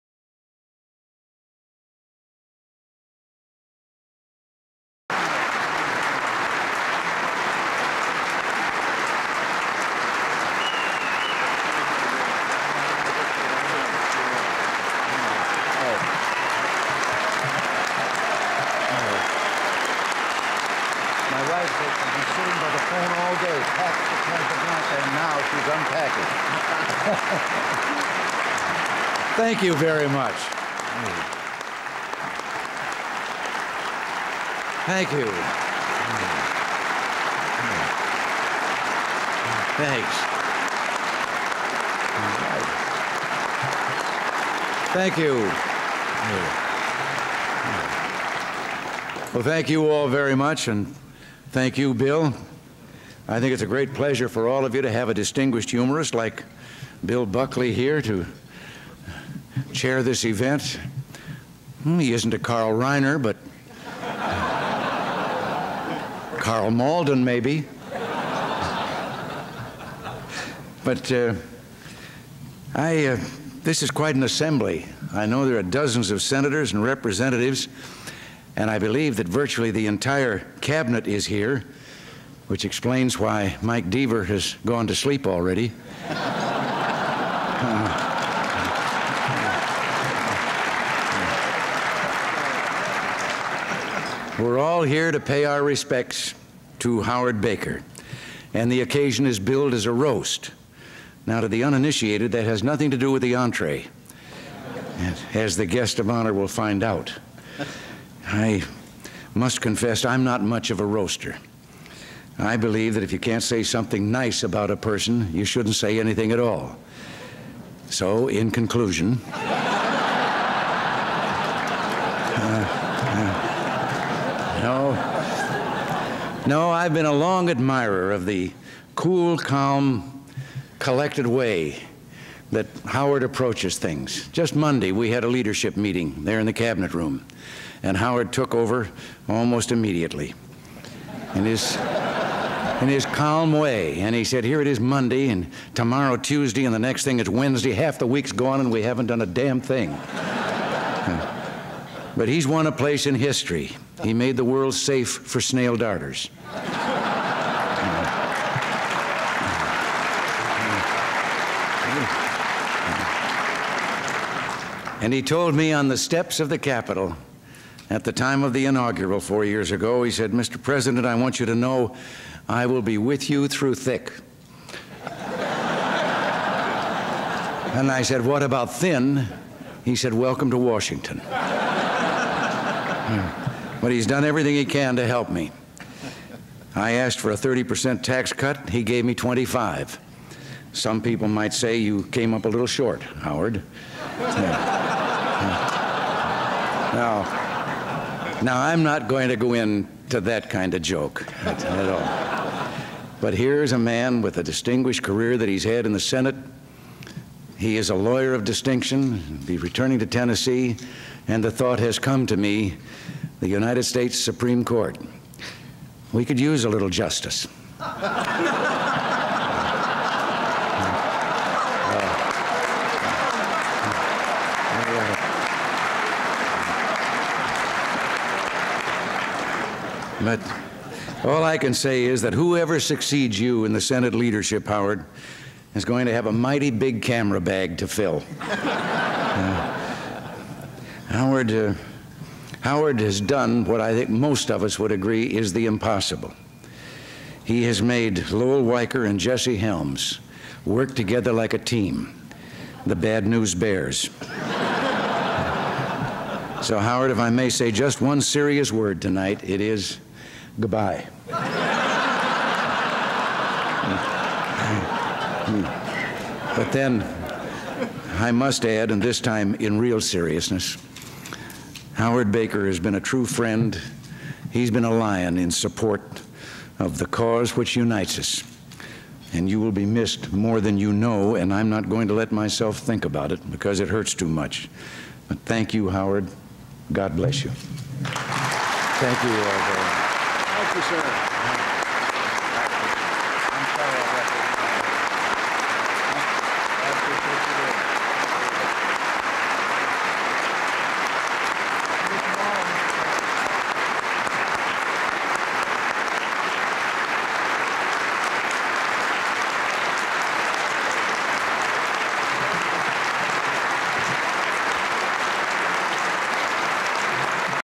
President Reagan's Remarks at a Howard Baker Roast on September 12, 1984
Full Title: President Reagan's Remarks at the Howard Baker Roast in the International Ballroom at the Washington Hilton Hotel in Washington, DC on September 12, 1984

Access Restriction(s):Unrestricted
Use Restriction(s):Unrestricted